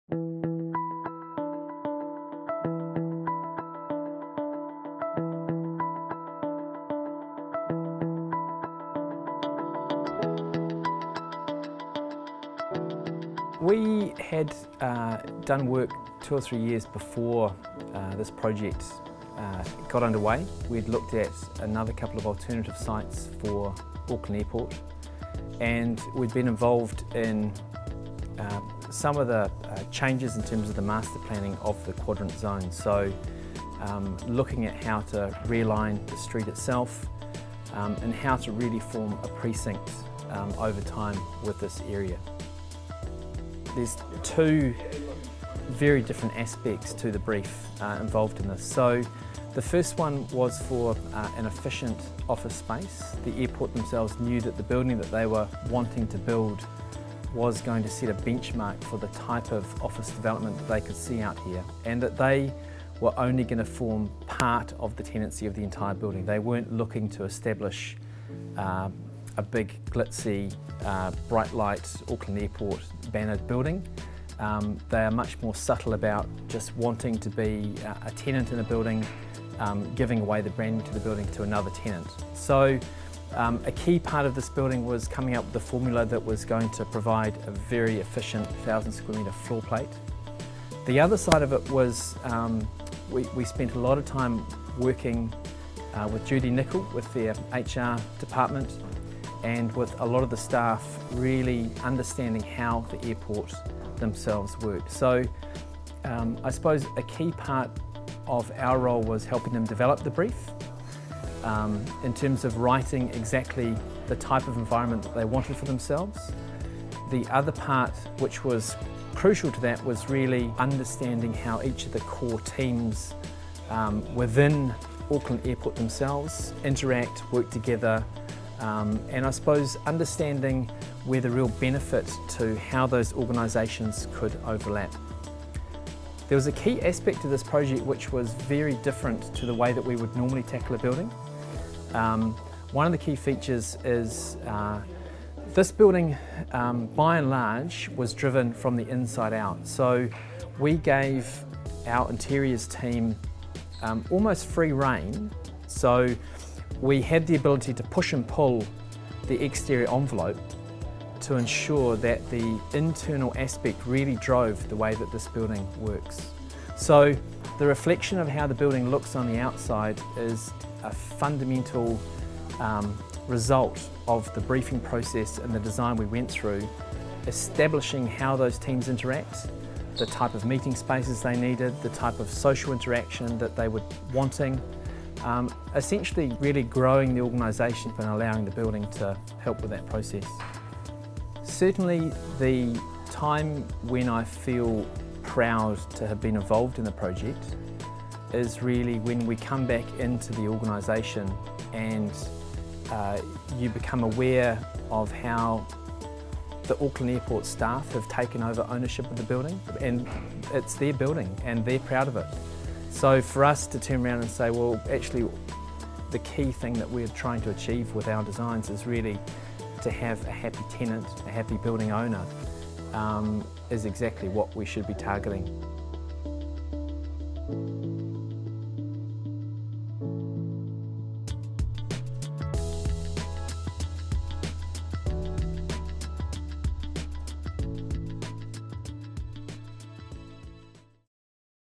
Interior design and layout determined the exterior of the new Quad5 office building
Trends Commercial Design Perspectives - Design from the inside out.
What was needed on the interior determined the exterior of the Quad5 office building.
Nick Moyes from Jasmax comments on the design process.